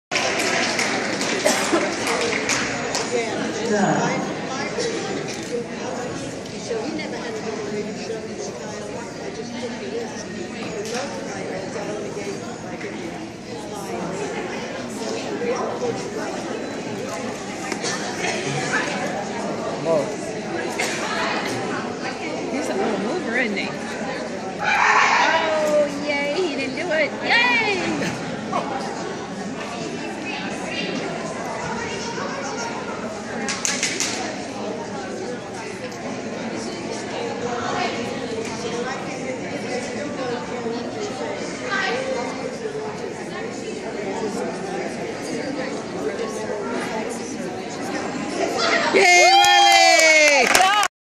Riley Master Jumpers
Riley running Master Jumpers with the Q and 1st place and 13 points. Finally a Q in Jumpers this weekend!